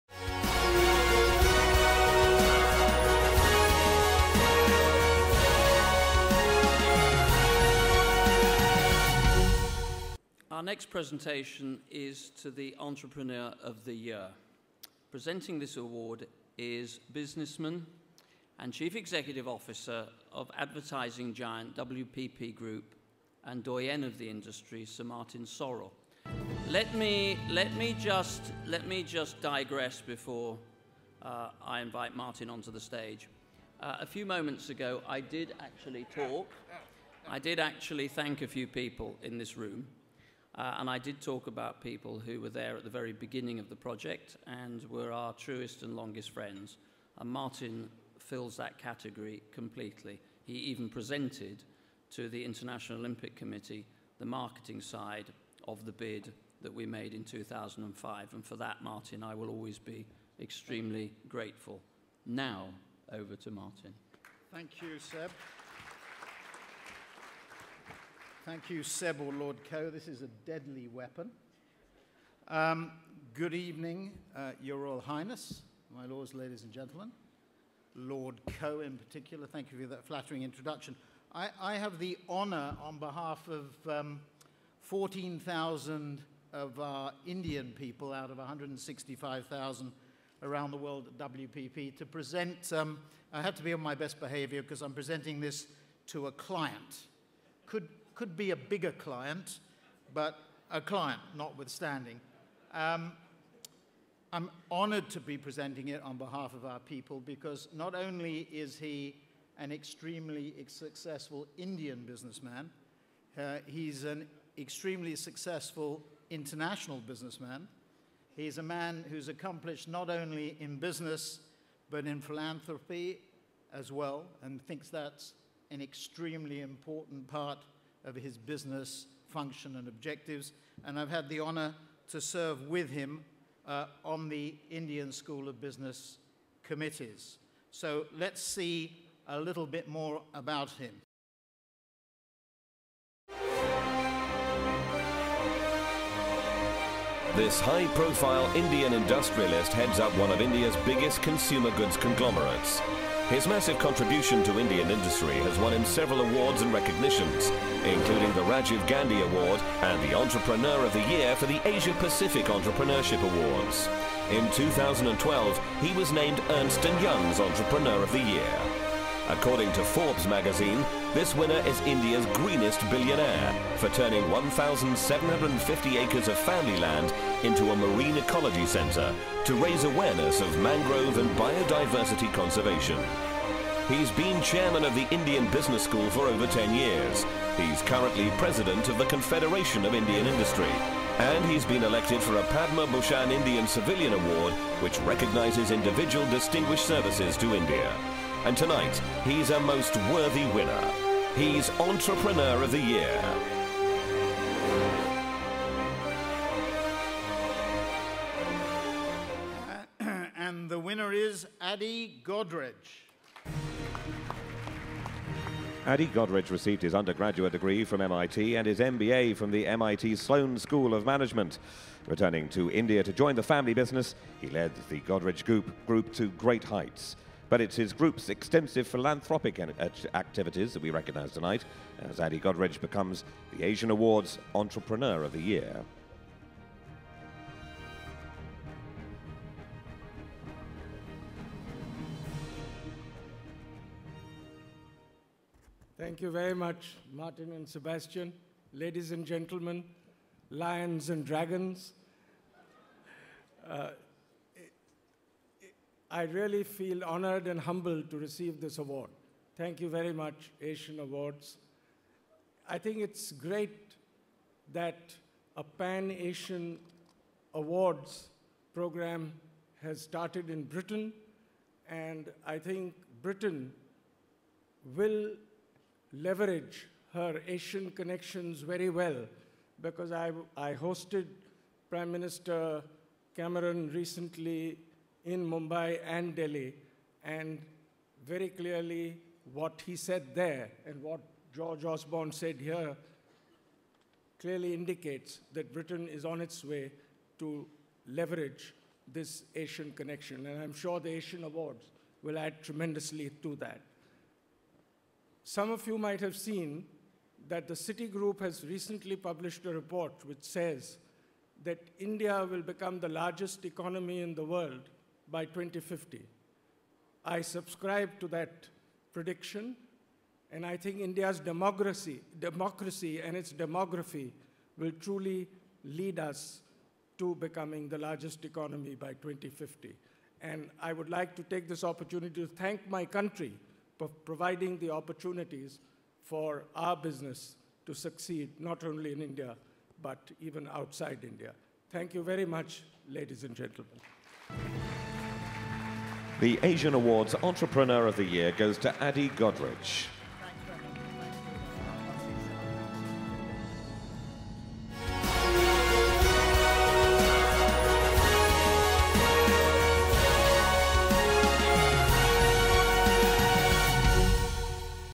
Adi Godrej Entrepreneur of the Year - The 3rd Asian Awards
Adi Godrej wins the Entrepreneur of the Year award at the 3rd Asian Awards